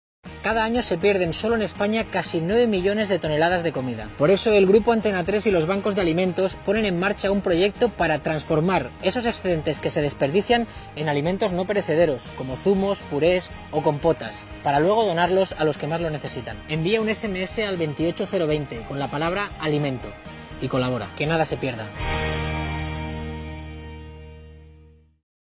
Jordi Évole Colabora contra el hambre
Los Bancos de Alimentos y el Grupo Antena 3 colaboran con una campaña protagonizada por Susanna Griso y Jordi Évole
El objetivo de este primer proyecto conjunto es transformar excedentes de alimentos frescos en productos no perecederos para su posterior donación.